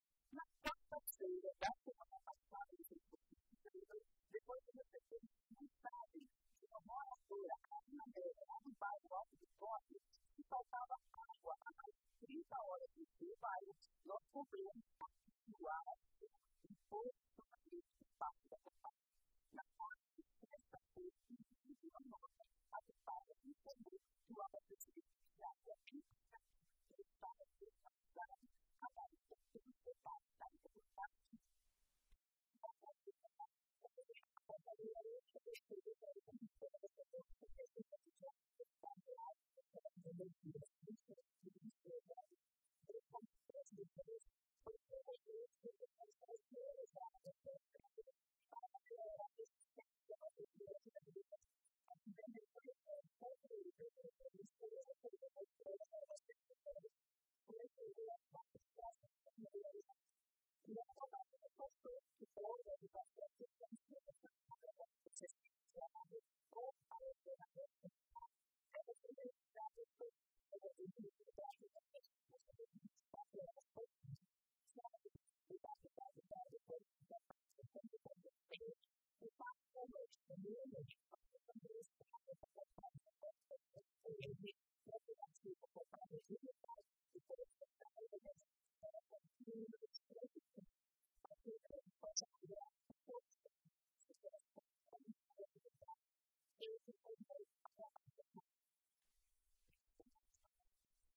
Copasa responde sobre falta de água no bairro Alto do Córrego
Nossa equipe fez contato com a companhia responsável pelo abastecimento de agua em Paracatu que nos respondeu através de nota.